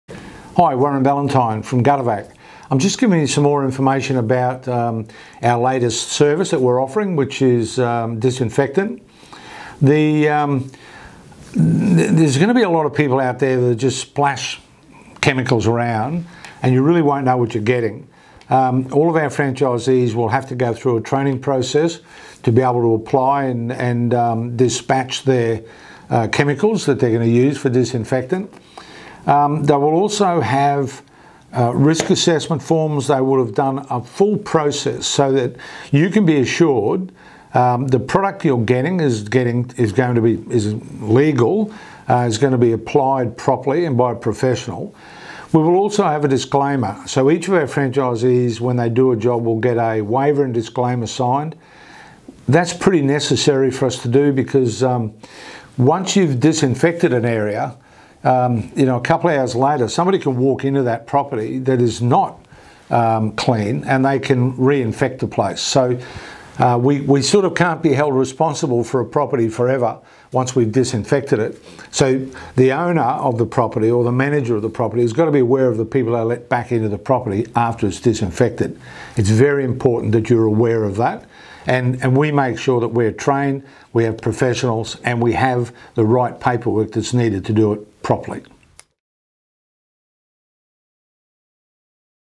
Gutter-Vac More information Disinfectant
Gutter-Vac is pleased to present the BactaEx™ disinfectant solution to our commercial and domestic customers' requests for disinfecting requirements.  Warren takes you through a few other very important items about disinfection.  Call Gutter-Vac on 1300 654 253.